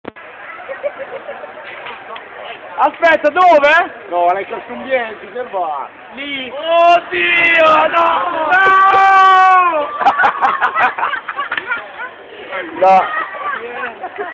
Bani nella siepe
bani versione johnny knoxville lanciato nella siepe nella devastante serata da peledo's